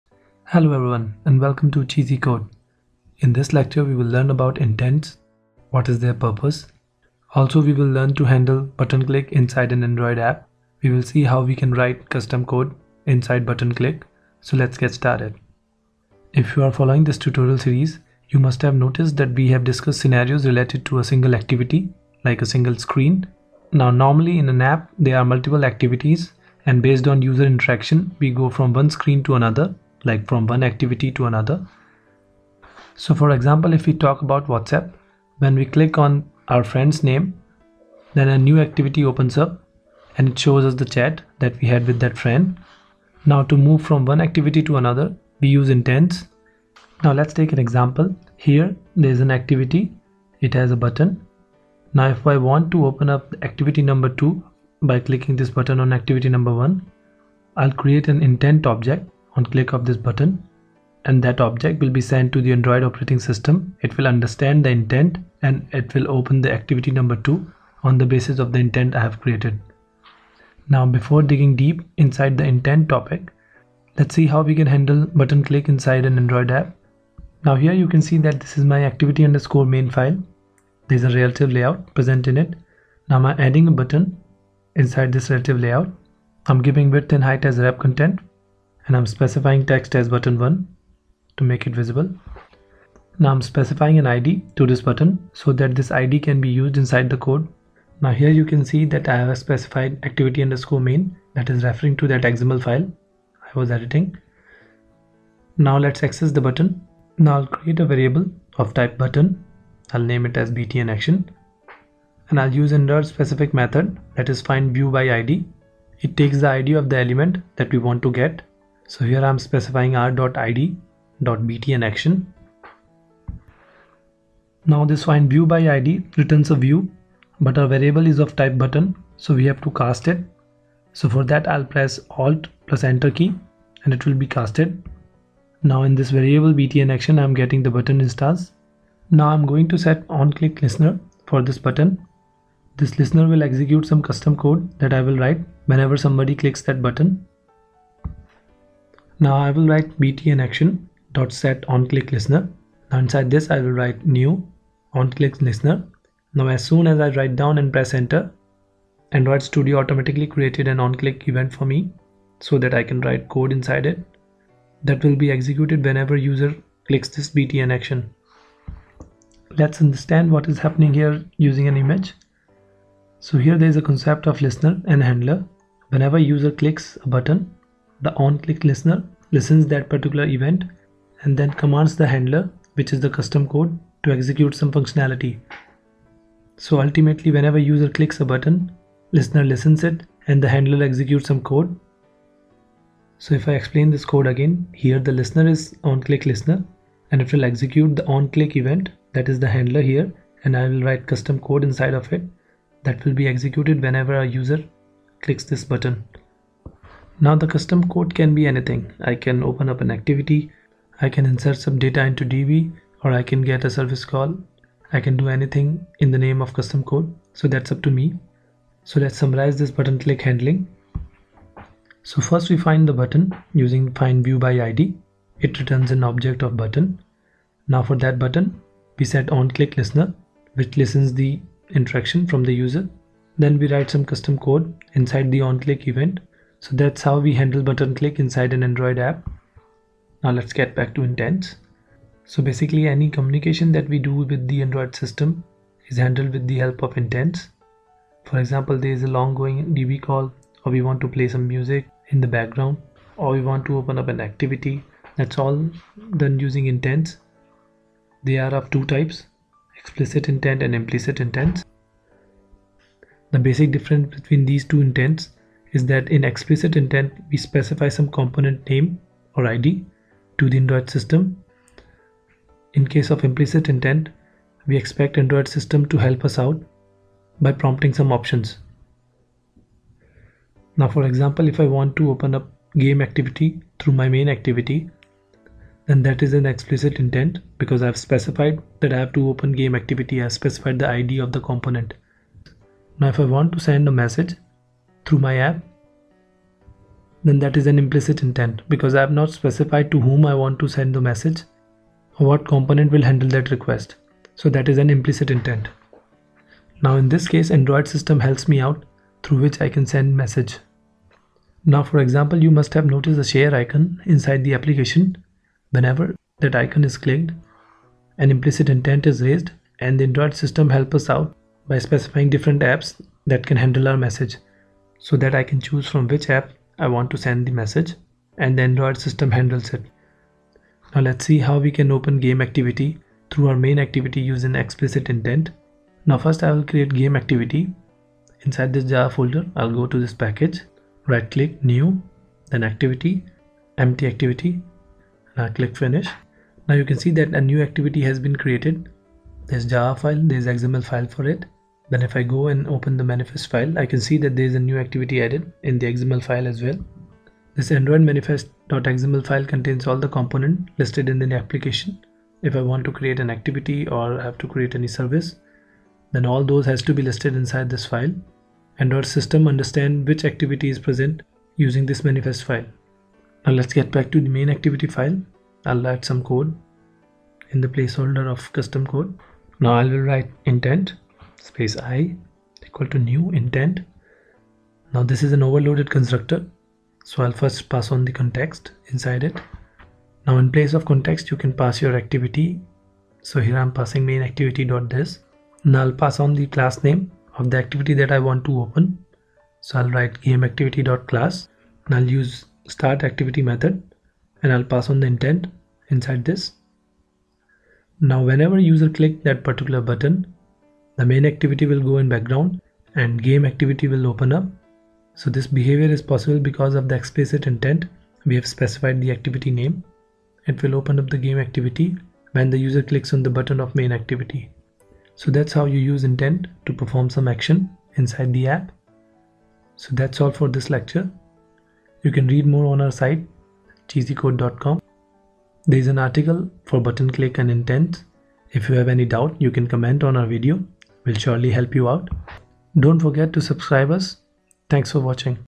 Android Intent Tutorial | Explicit and Implicit Intent With Examples - #9
In this Android tutorial you will learn about Android Intents i.e. Explicit Intent and Implicit Intent. Differences between Explicit and Implicit Intents with examples. You will learn how to handle button clicks in Android. This is the 9th video in the series of Android App Development Tutorials.

You can refer this series for other android tutorials. Just follow the series and start creating your android app.

For more info and source code - Visit below link

We are social. Follow us at -